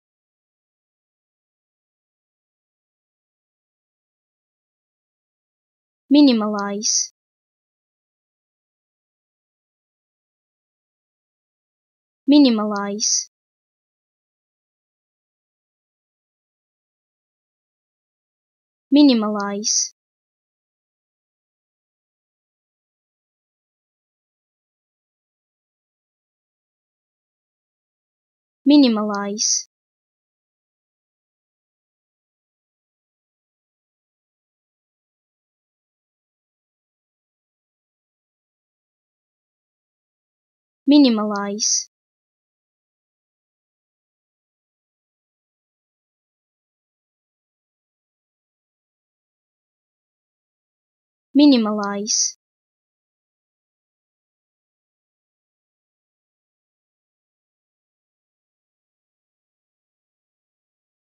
How To Say Minimalise
Learn how to say Minimalise with EmmaSaying free pronunciation tutorials.